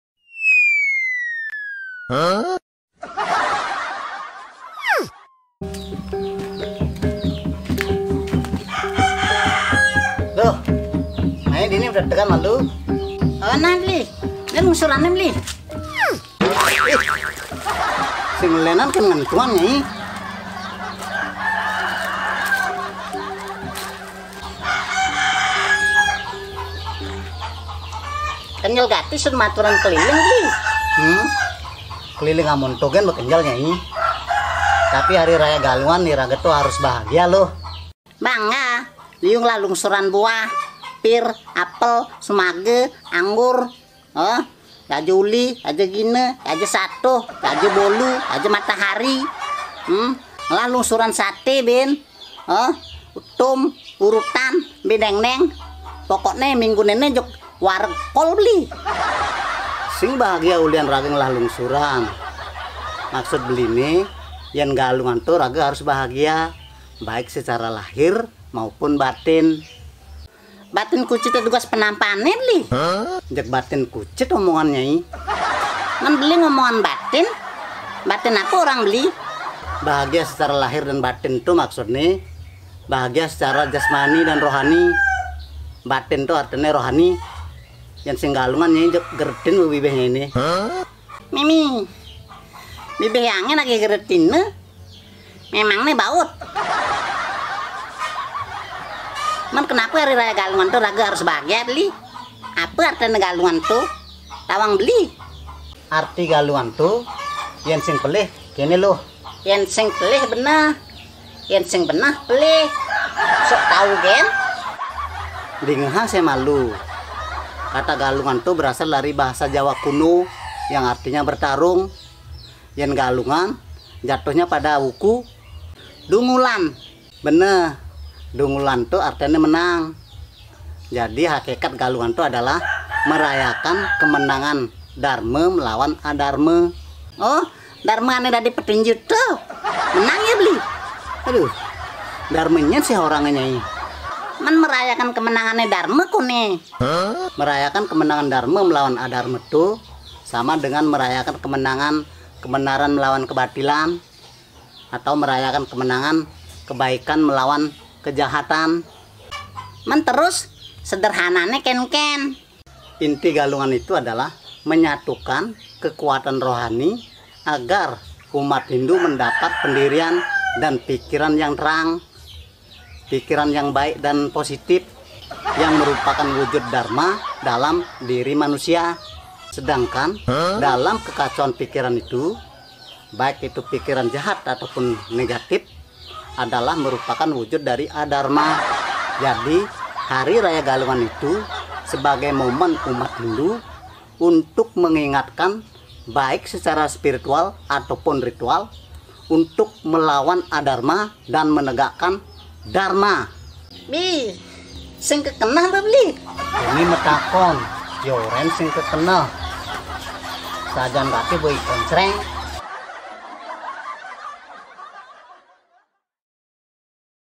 Lawak bali ngakak, makna Hari raya Galungan - tut bradut
Banyolan edisi Galungan, Hakikat Galungan adalah merayakan kemenangan dharma melawan adharma